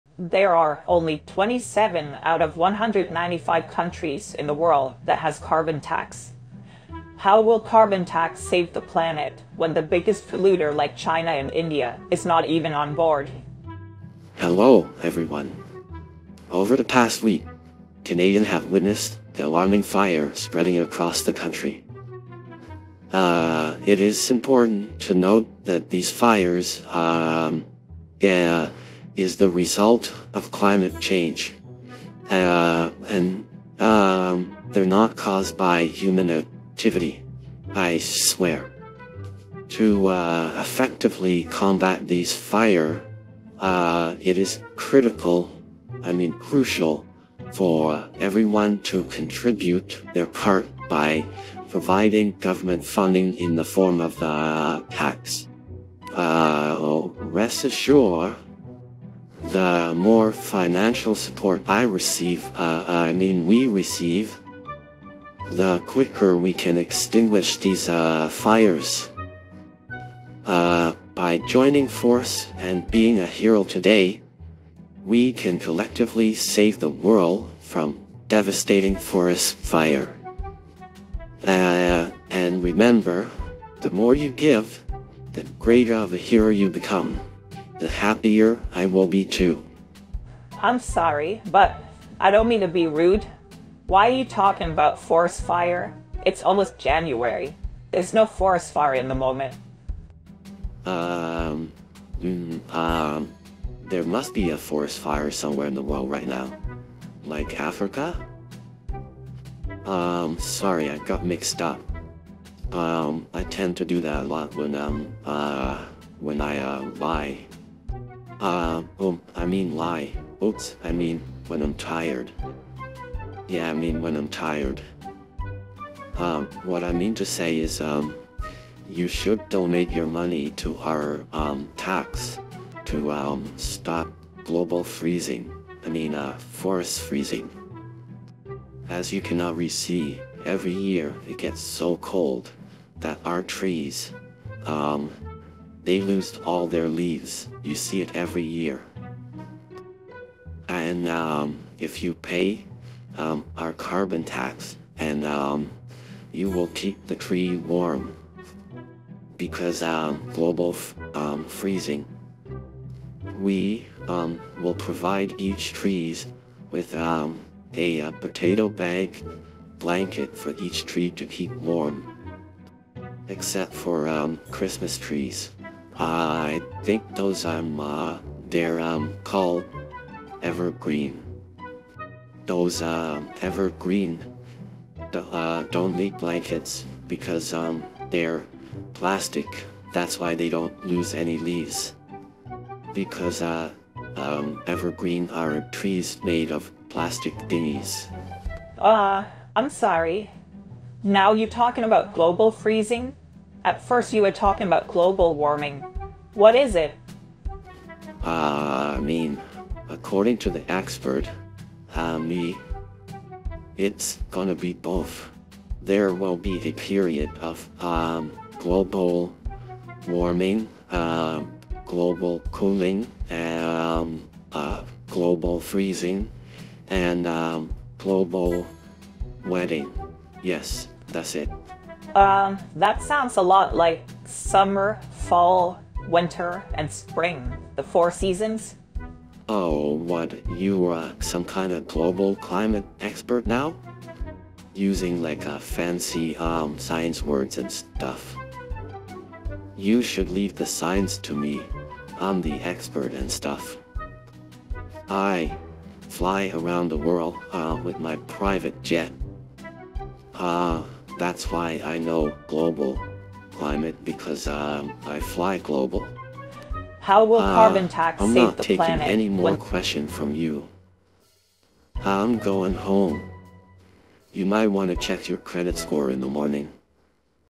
Justin Trudeau The Global Climate Expert (Edited Satire)
So here we have Justin Trudeau, the man on a mission to convince all us Canadians to cough up our hard-earned cash for his precious carbon tax. And let me tell you, folks, he's not holding back when it comes to spinning some wild tales to the press. I mean, seriously, this guy's imagination knows no bounds! But hey, let's not forget, this is all just a little satire skit, okay? Don't go taking it as gospel truth or anything. If you want the real deal on climate change, go ask the experts. They'll give you the lowdown. But in the meantime, let's sit back, relax, and enjoy the show as Trudeau tries to sell us his carbon tax fairy tales.